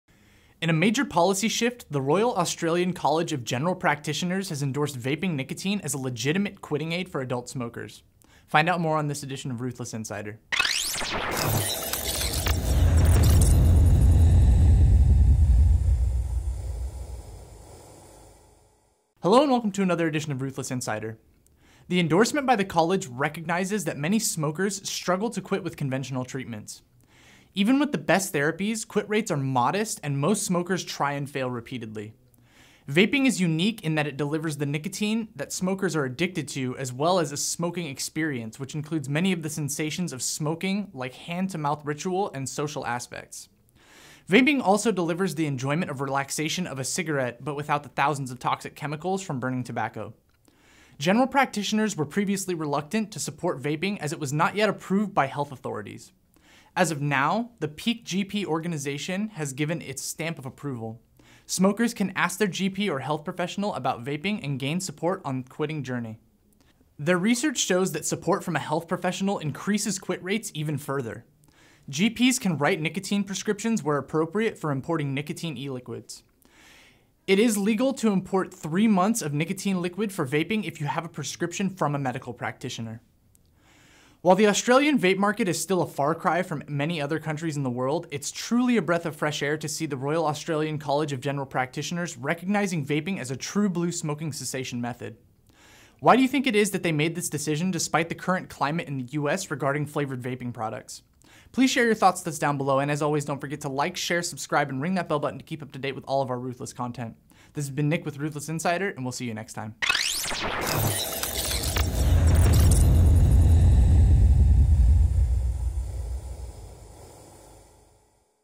Ruthless Insider: Australian GP body now supports vaping
In a major policy shift, the royal Australian college of general practitioners has endorsed vaping nicotine as a legitimate quitting aid for adult smokers. Find out more on this edition of Ruthless Insider.